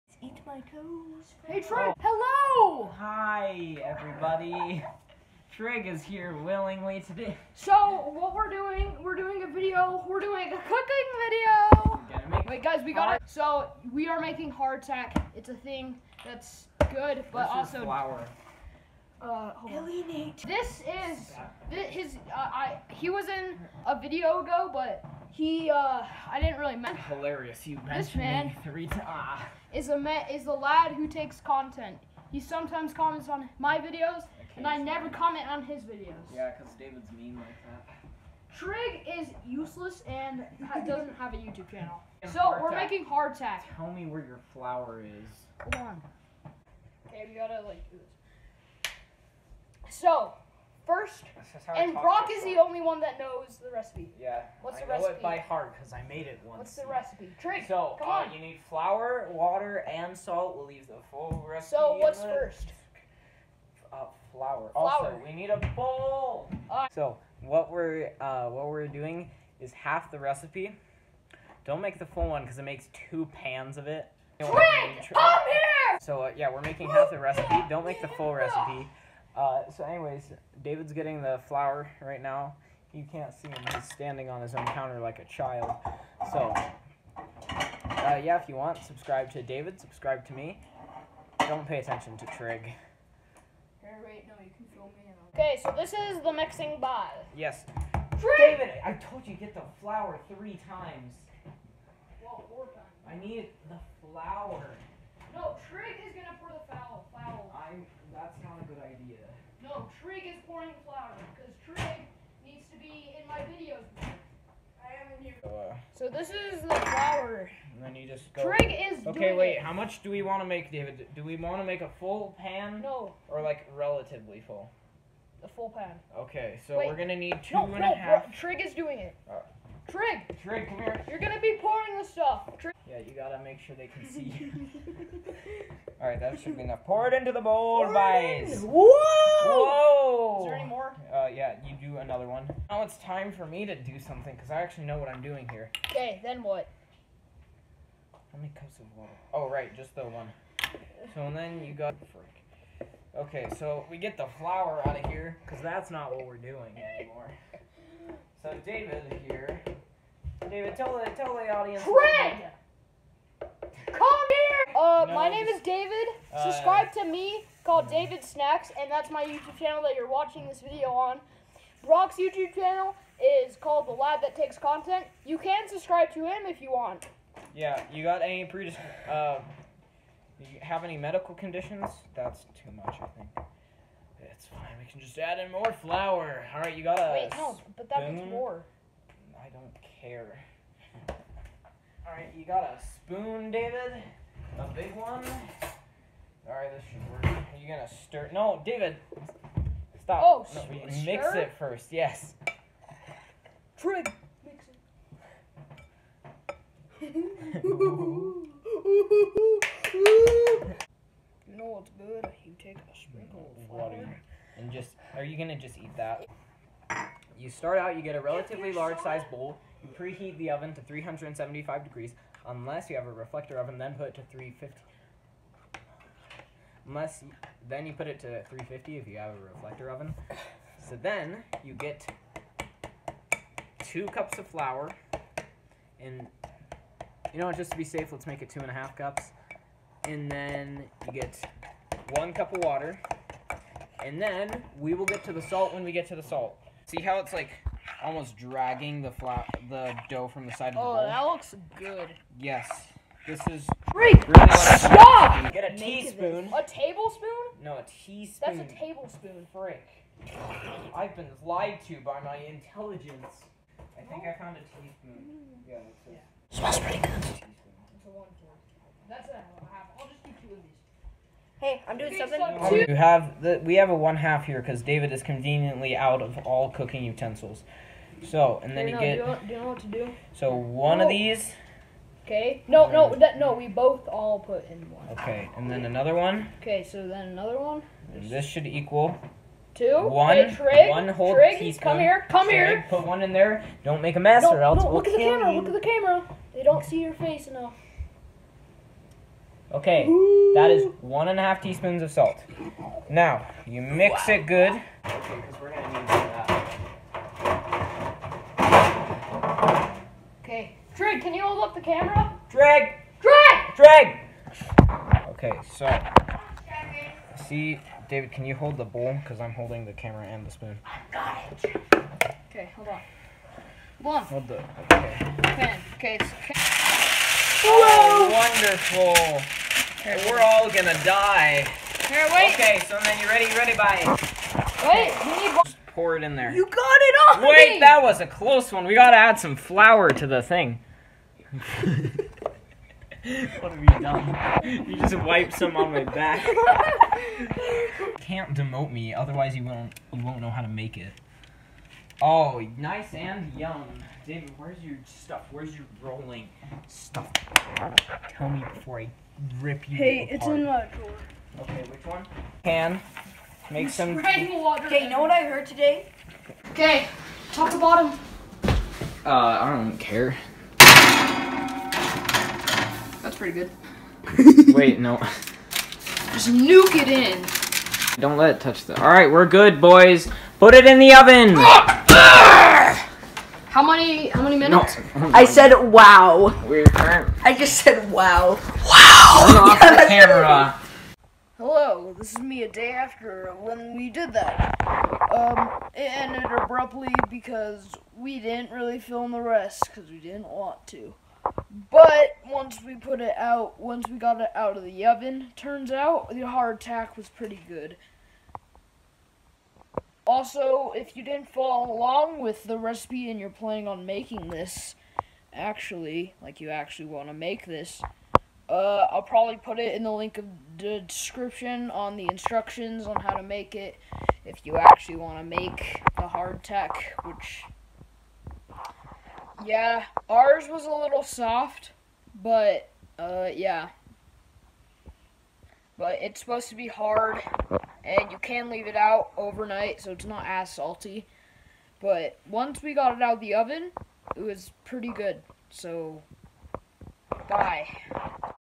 Me and my friends made hardtack
I don’t know how to put links but brocks channel is
The lad That takes content

The recipe for hardtack is
Pre heat a oven for 375 degrees then
Put 2 cups of flour into a relatively large bowl
Then 1 to two cups of water
Mix it until it turns into a dry doe
Then two teaspoons of salt
Then put it in oven for 30 minutes then take it out.